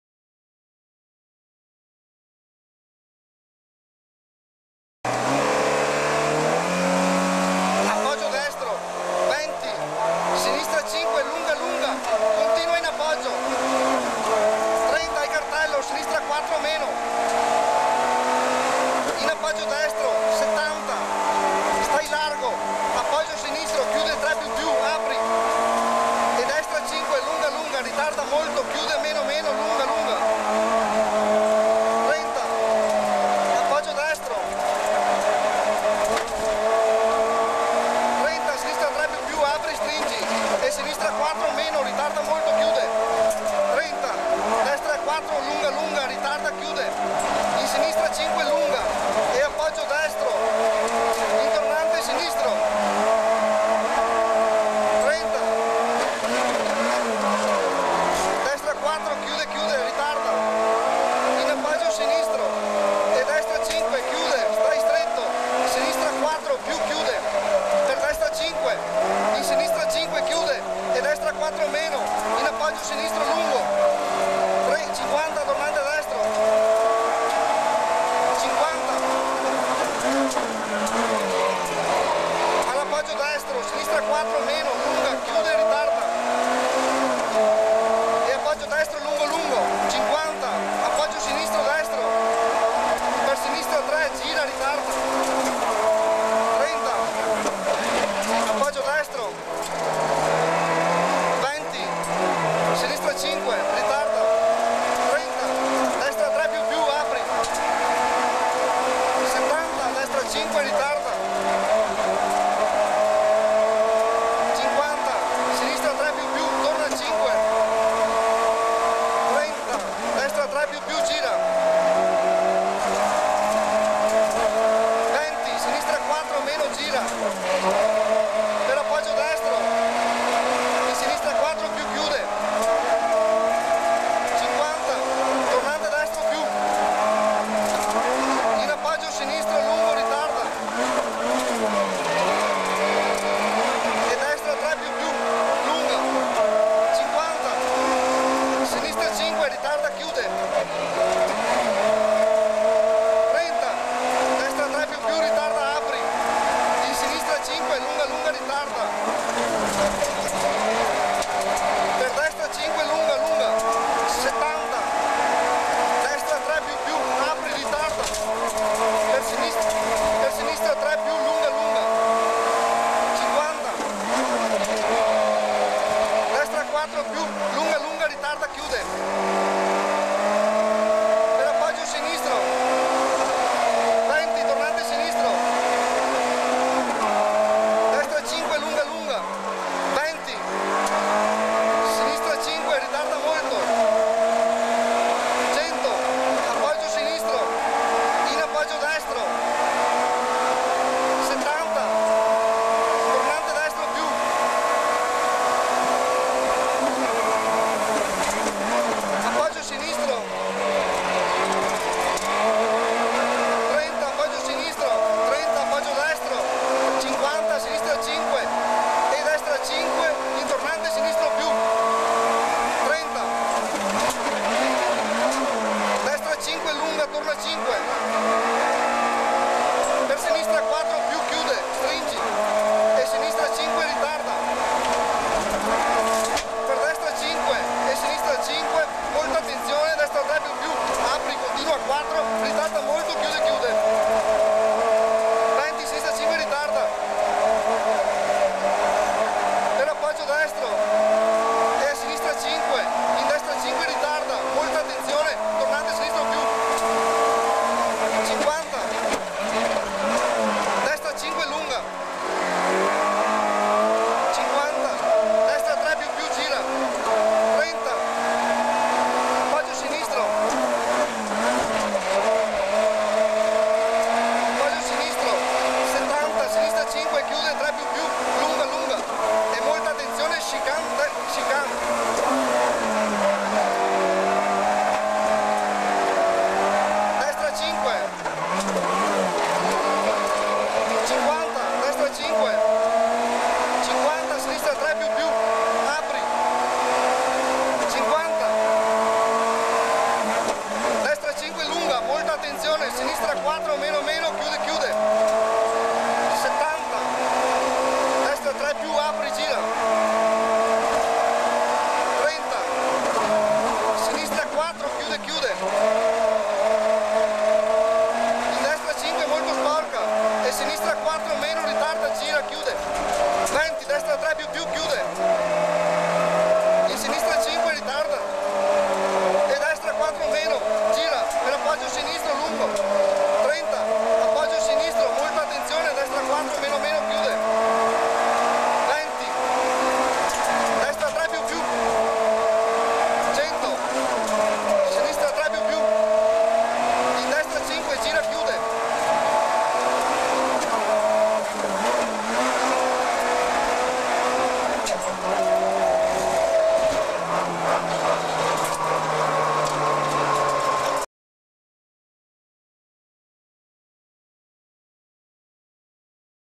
Cameracar Zamparini-Guidolin al rally Alpi Orientali del 1994 Video Si
Stasera saliamo sulla piccola Peugeot 106 xsi della giovane promessa rallistica Patrick Zamparini navigato da Remo Guidolin all'alpi orientali del 1994. pronti partenza via. buona visione. video si